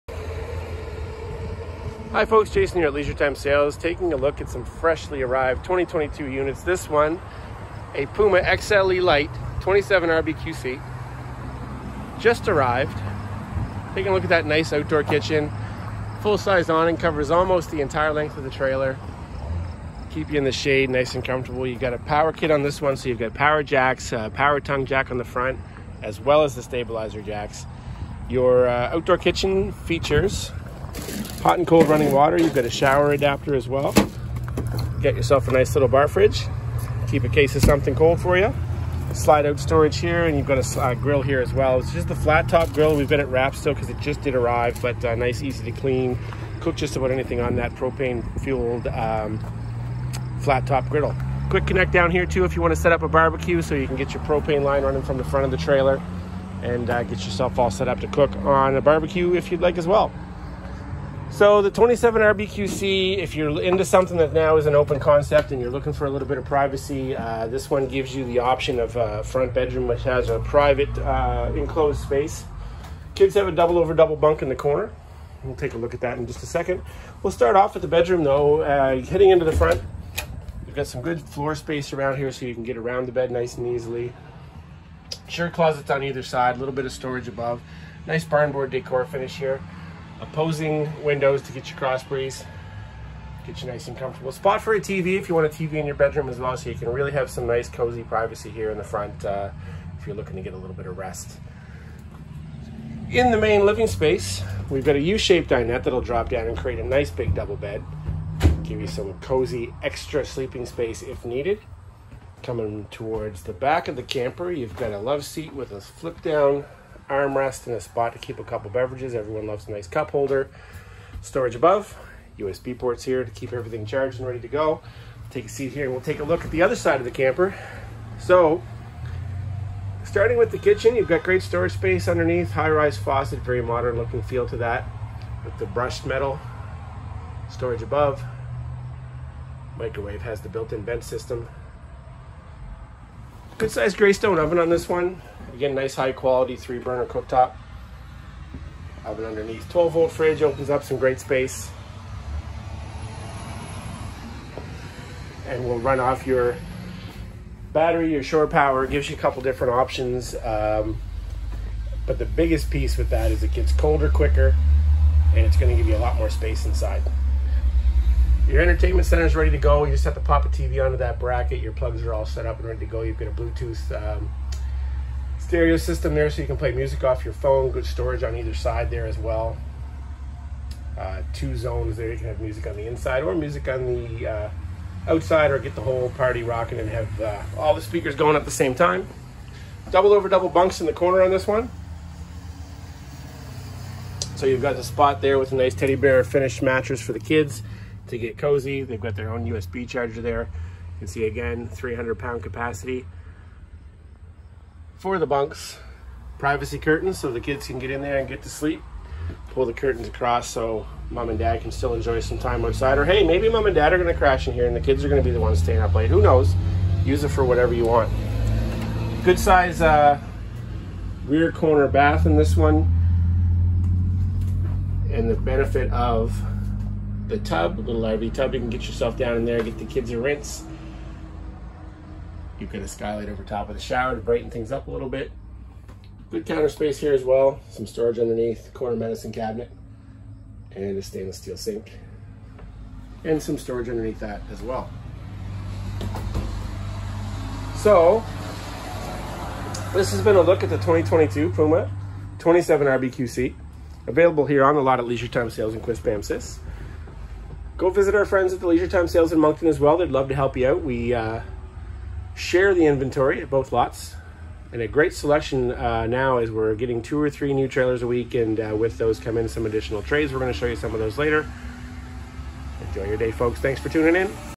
2022 Palomino Puma XLE 27RBQC
Great for the family on the go, this camper features an enclosed bedroom for the adults, double over double bunks for growing kids and a great sized social space with a large slide. An outdoor kitchen and large awning lets you have your fun outside!